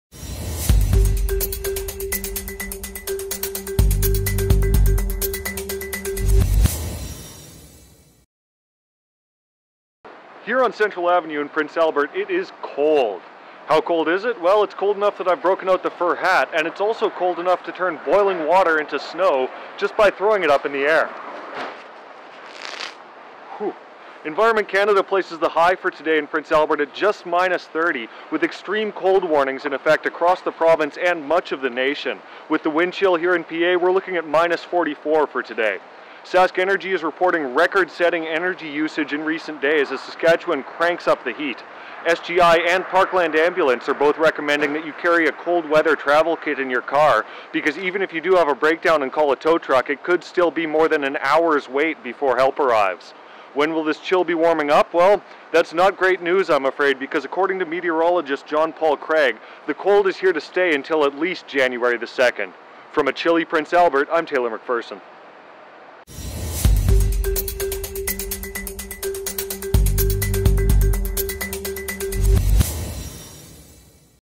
Arctic weather grips Prince Albert
Reporter Taylor MacPherson talks about the Arctic deep freeze gripping the nation, when it might let up, and has some fun with boiling water.